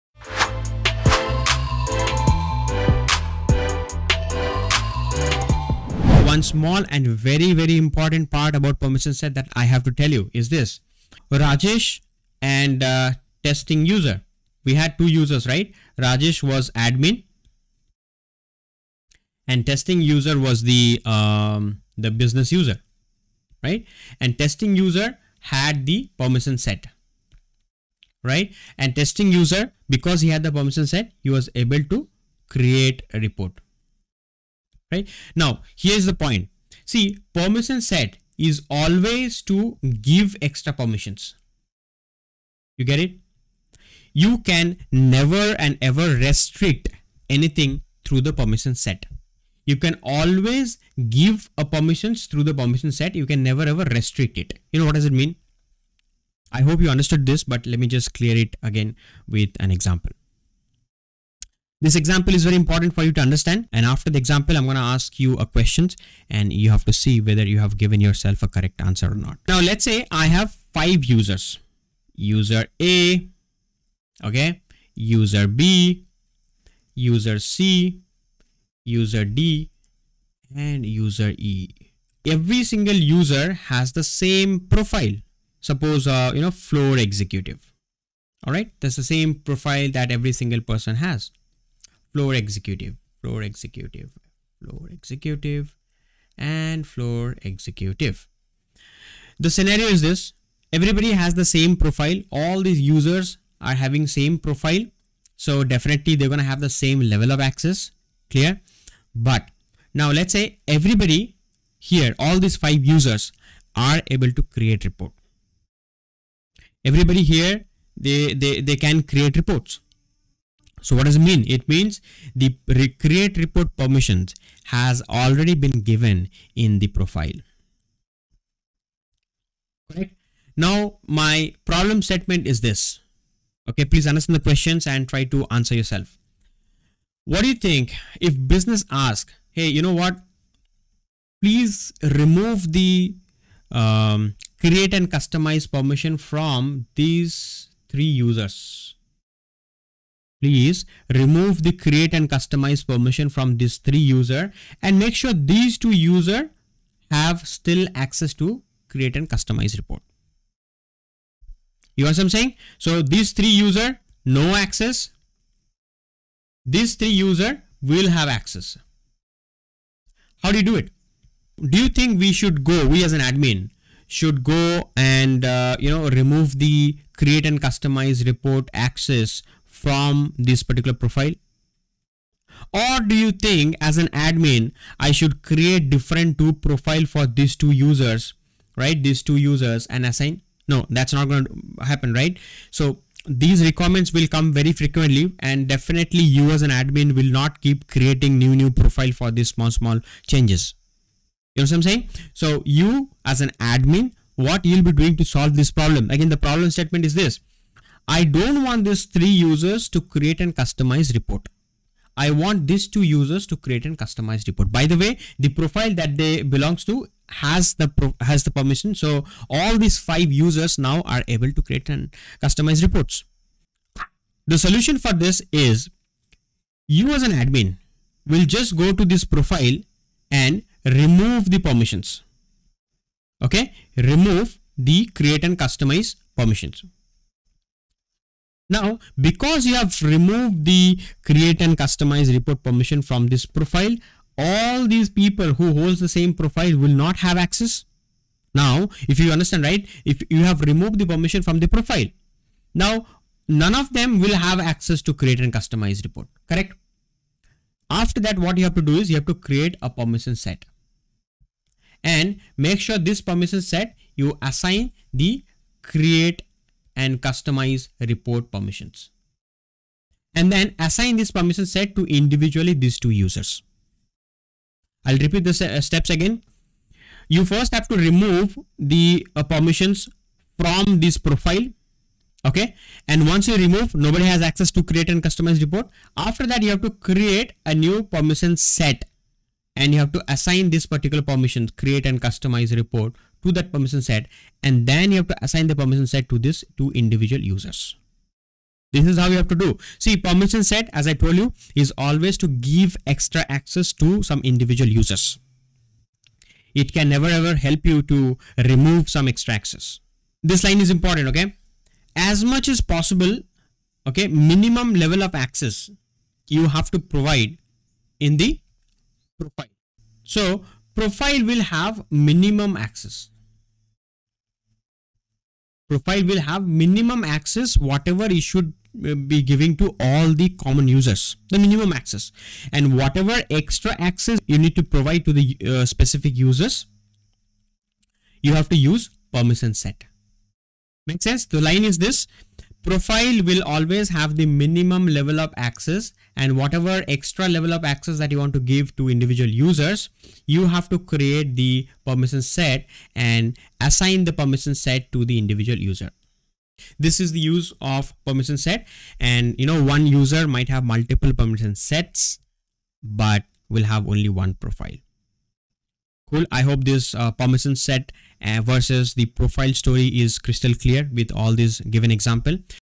Business Scenario related To Permission Set || By Rajesh Chatterjee || Delipat.
Understanding permission sets with real-world business example

In previous articles/videos, we discussed permission sets and the creation and assignment of permission sets. Now, let us understand permission sets better with a business scenario.

A company has five sales executives- User A, User B, User C, User D and User E. All the five users have the same profile- Sales executives.  The sales executive profile grants permission to all five users to create and customize reports. As a result, all five users can create and customize reports in Salesforce.

But, the company wants to change the access. It does not want Users A, B and C to create and customize reports. The company wants only the users D and E to create and customize reports in their Salesforce org.

Now, as a system admin, how will you configure the settings in Salesforce?

Will you create permission sets to restrict the access for users A, B and C?
That is not possible, right?

Because permission sets cannot restrict permissions. They can only give permissions.

So the System admin will give minimum access through profile and open up the accesses with permission set for specific user.
Points to remember about permission sets

Permission sets can grant additional or extra access to an individual user. They act as additives.
Permission sets cannot restrict access for any user. They can only be used to extend the functionality of users without changing profiles.
The settings and permissions available in permission sets are the same as those in profiles. But, they can be assigned to individual users.
One user can have multiple permission sets assigned to him.
A profile is mandatory for a user, not the permission set. Hence, a user may or may not have permission sets assigned to him.

Now, how will you configure the settings in Salesforce for the company discussed earlier?

In the above discussed business scenario, you should remove the access to create and customize reports for the Sales executive profile. Once you remove the access, no user with a sales executive profile will be able to create and customize reports.

After removing the access from the profile, you can create a permission set with the access to create and customize reports. That permission set can then be assigned to Users D and E to grant them access to create and customize reports.

In this manner, you can combine permission sets and profiles to grant the required level of access to the users.

For object and field-level security, profiles can be used to give baseline or minimum access to users and extend the access using permission sets. Similar to record level security, where organization-wide defaults are used to give baseline access, and sharing rules, manual sharing and role hierarchy are used to extend the record access.

I hope this business scenario helped you understand permission sets in a better manner. Let us test the permission sets we created in the next article/video.